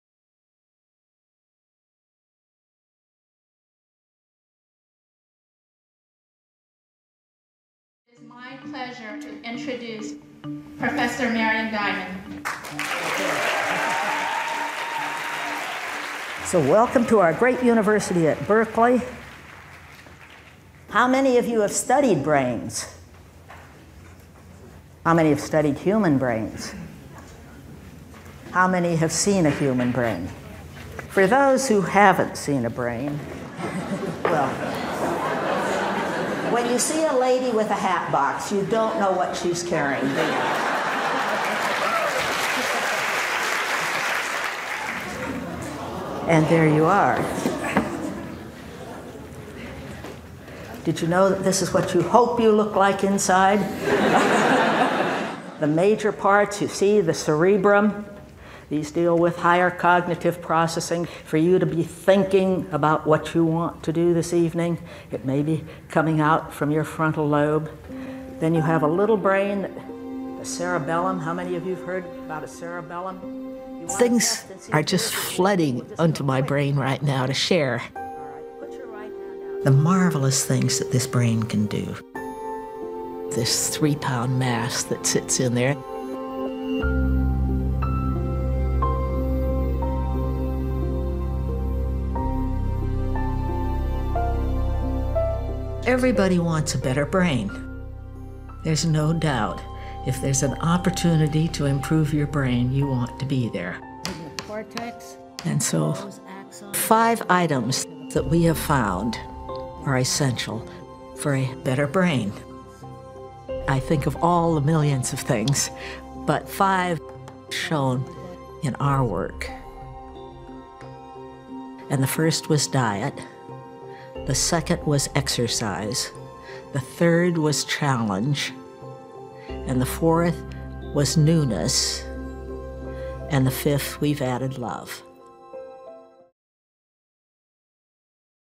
Marian Diamond on Building a Better Brain
Diet, Exercise, Challenge, Newness, And Love